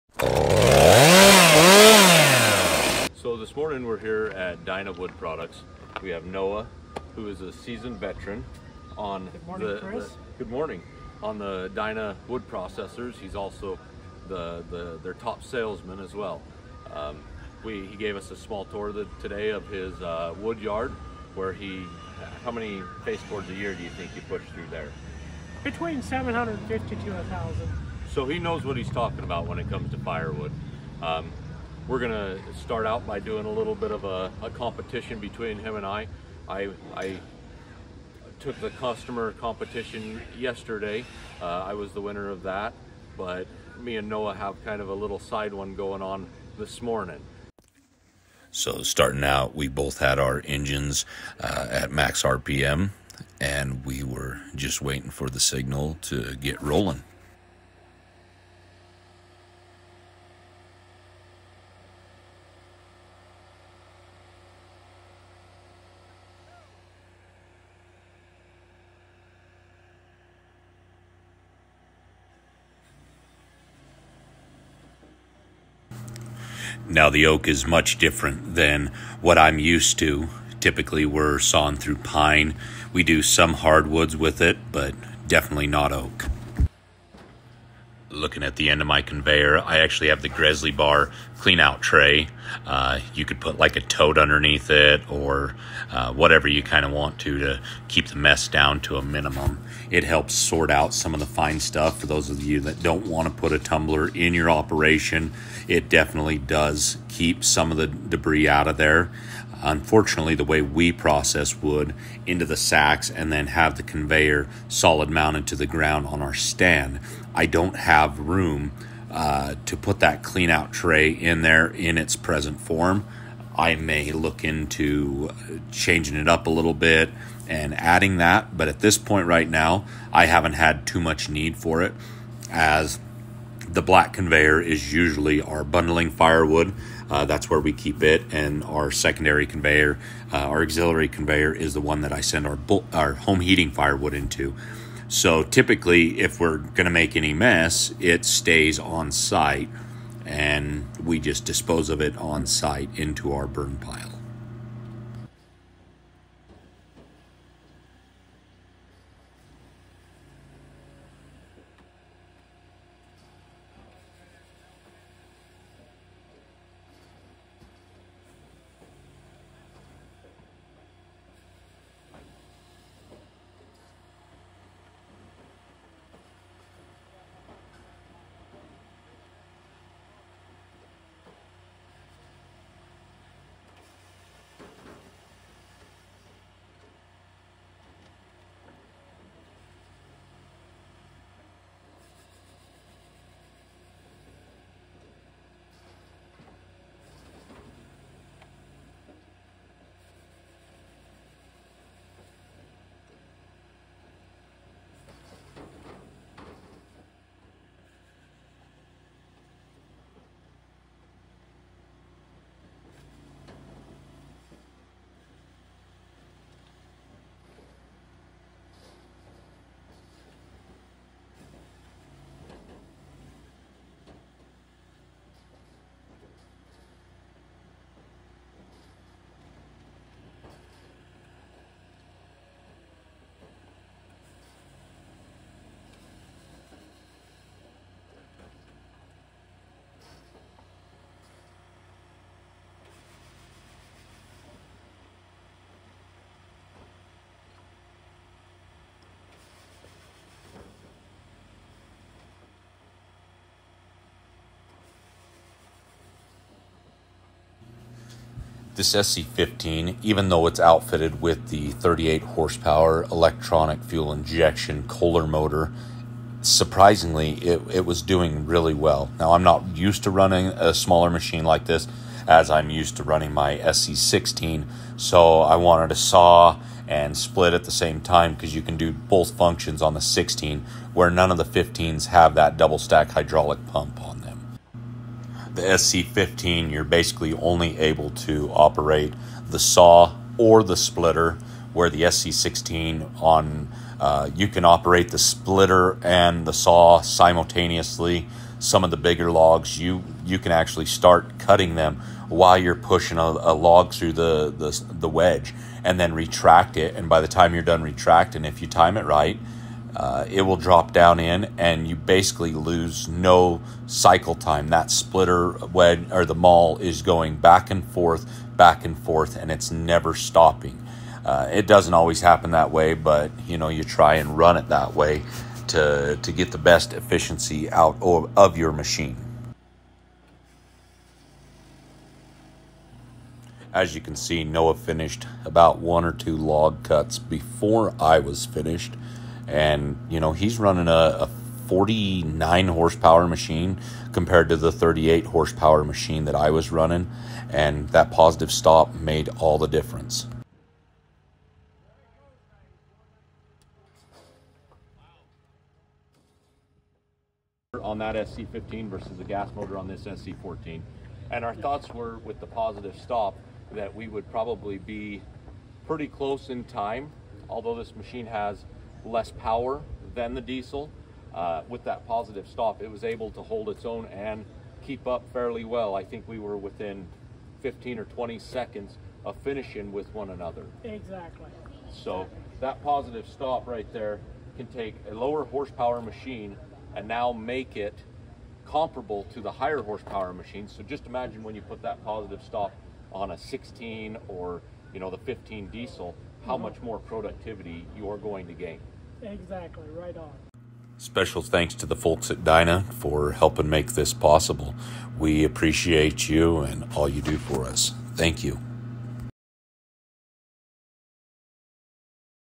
Comparing Dyna processors
We compared the Gas vs diesel SC-15 firewood processors. Enjoy!!!